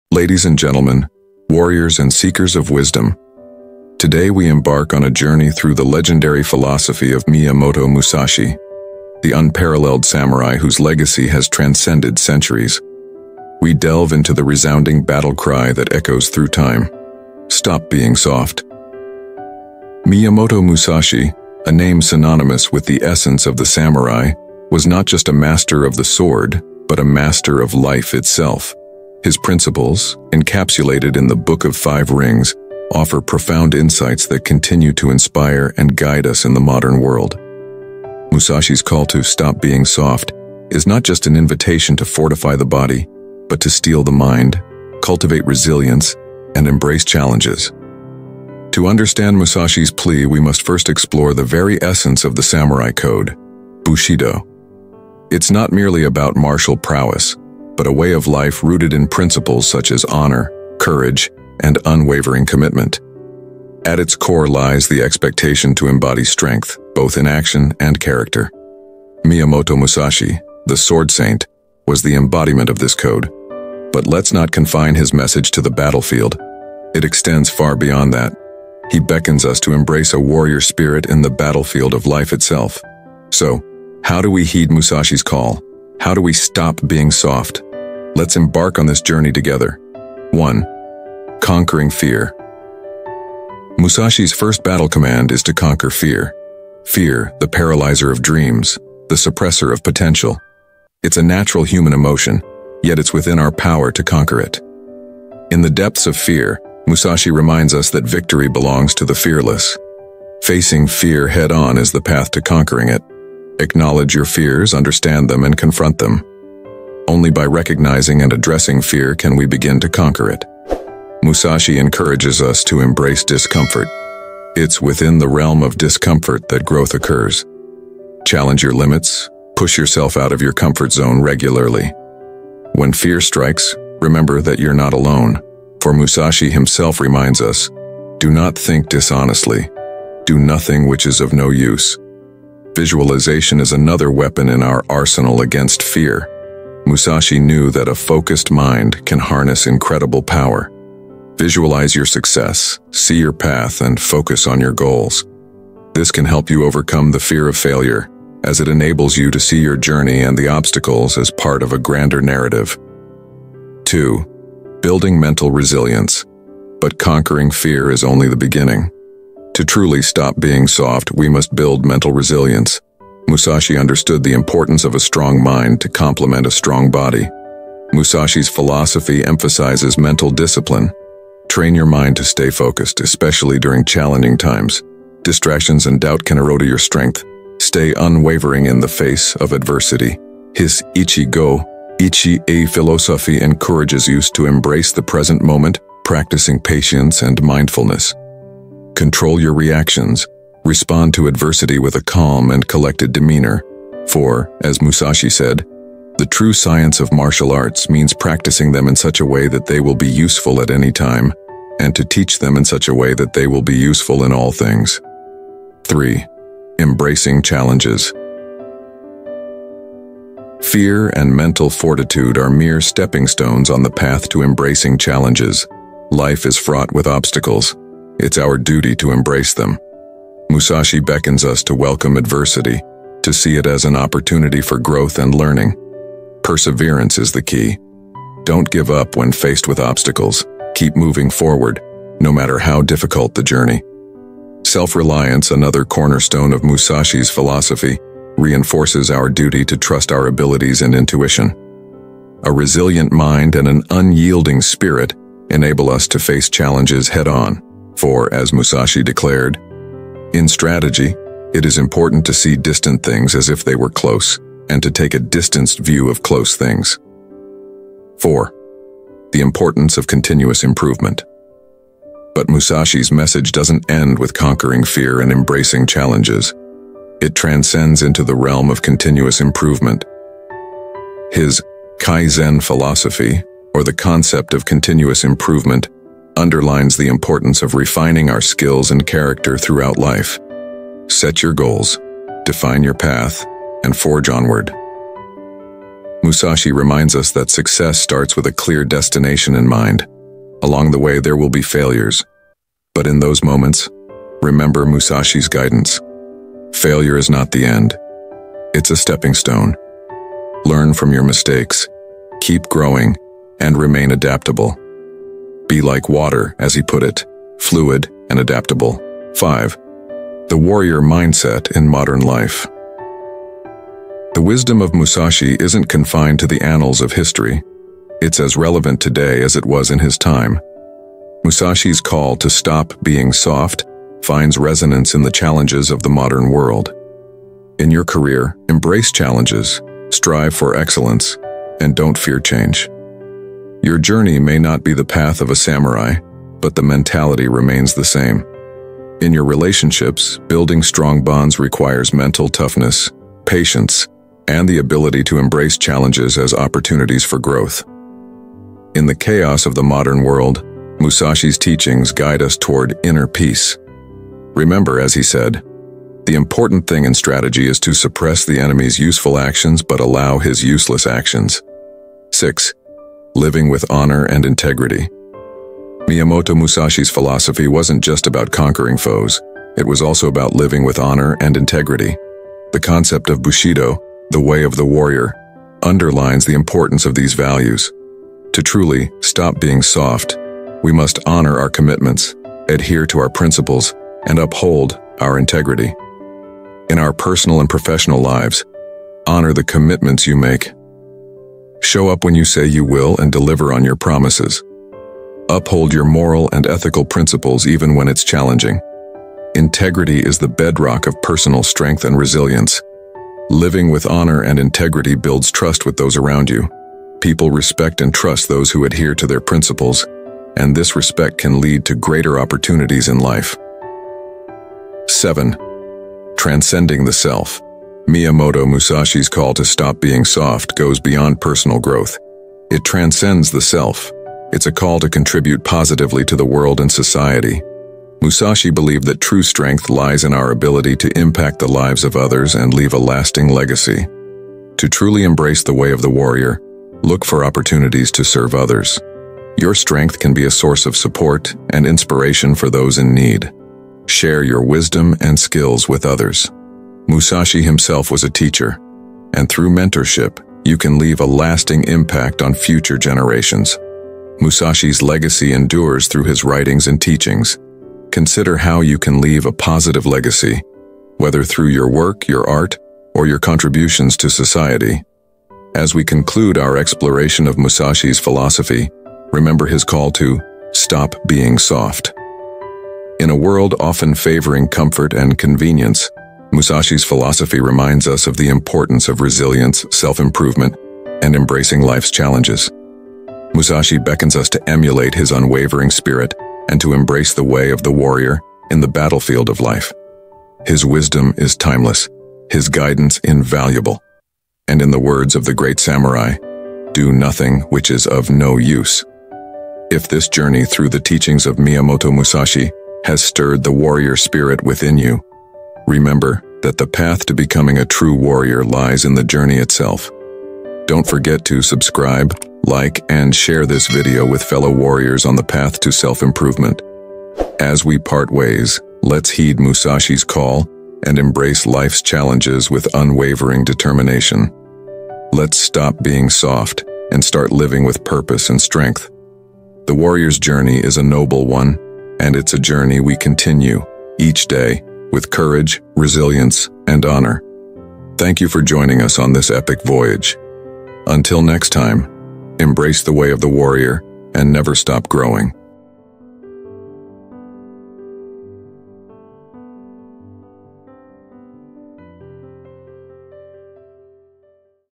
CONTROL YOUR EMOTIONS | Miyamoto Musashi
"Welcome to an epic journey through the annals of history and the heart of a warrior's spirit! In this video, we delve into the timeless wisdom of Miyamoto Musashi, the legendary samurai who not only mastered the blade but also the art of life itself. Musashi's call to 'Stop Being Soft' isn't just an invitation to toughen up; it's an awakening of courage, resilience, and honor.

Join us as we explore the profound teachings of Musashi and how they transcend time and space, empowering us to face life's challenges with the spirit of a true warrior. Learn how to conquer fear, build mental resilience, and embrace adversity with unwavering determination.

If you're ready to embrace the warrior within and heed Musashi's timeless wisdom, don't forget to like this video, share it with fellow warriors, and subscribe to our channel for more enlightening journeys into the world of self-improvement and wisdom.

The call to 'Stop Being Soft' awaits you, my fellow warriors. Will you answer it?"